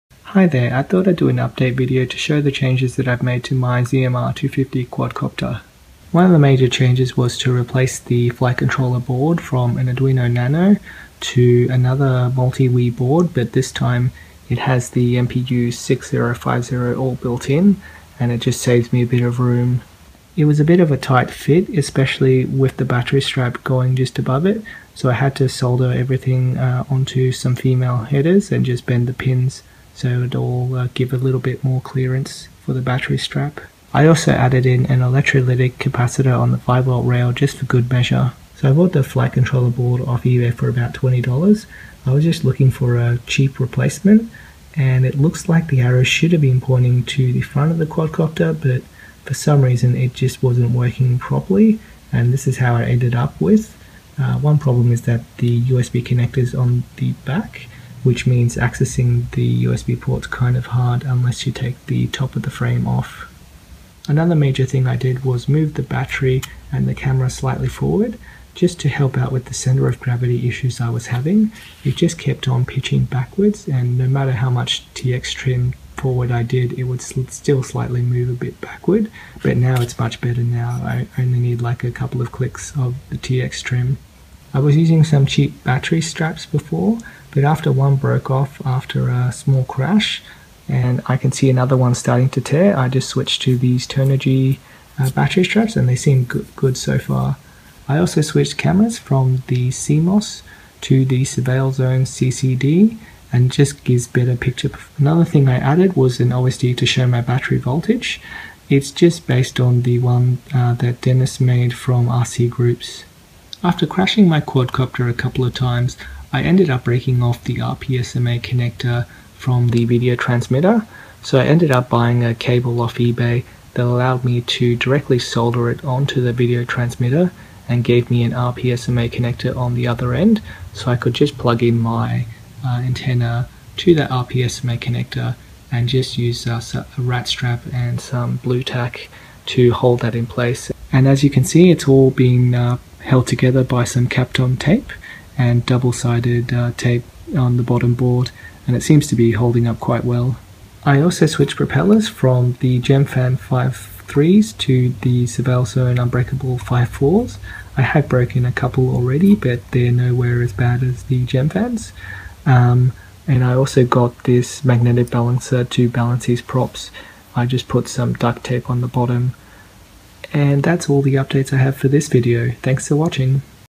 My ZMR250 Quadcopter Build Update
Made some changes to my ZMR250 quadcopter -

MPU6050w/USB MWC ATMega328p 6 Axis Gyro/accelerometer Control Sensor Flight controller $13

U.FL/IPX to RP-SMA Female Nut Bulkhead Pigtail 1.13 Cable For PCI Wifi Card $1 - cut off the U.FL part and directly soldered to Video TX:

Sony Super HAD CCD 600TVL FPV IR Block Camera 2.8mm Lens $26

Fiber Magnetic Propeller Balancer Prop for Quadcopter FPV Helicopter Airplane $4 from Ebay

Edit 29 Sep 15: For props, you can also try the DAL 5045 Bullnose props (not shown) or the Gemfan 5045 BN, they are much quieter and offer more lift than the 5040 unbreakables, you can feel more lift with less throttle needed, so worth getting some but just be aware that they can make the quad harder to control in wind, I think I'll stay with the 5040. Need to test the BN out more though.

Gemfan 5045 Bullnose too $3